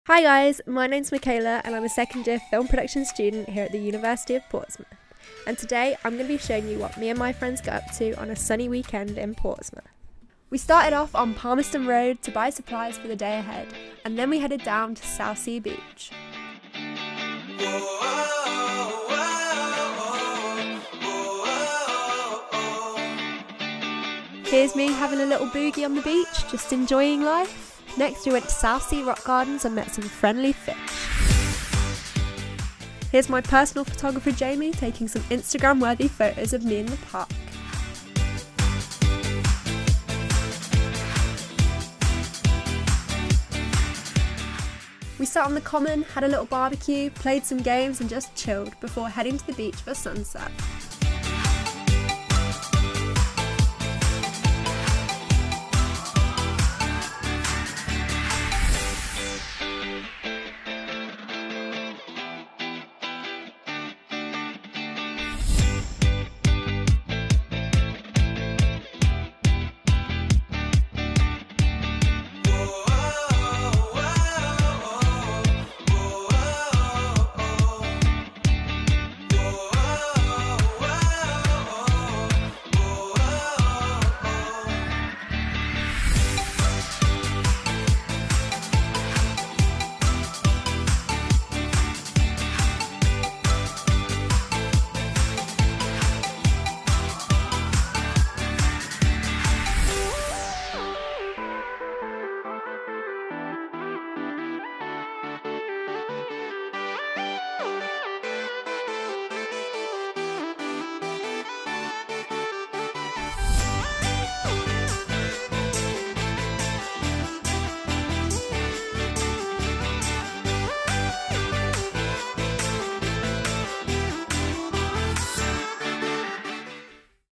Spend a Saturday with me in Portsmouth | Student Vlog
Ever wondered what students get up to on the weekend in Portsmouth? Michaela, a second-year Film Production student shares a sunny Saturday in Portsmouth with you. She visits Palmerston Road, the Rock Gardens, Castle Fields, and Southsea beach to catch a beautiful sunset.

Discover more about student life in Portsmouth: uni.port.ac.uk/3xXPHUL

Chat to current students to find out more about studying & living in Portsmouth: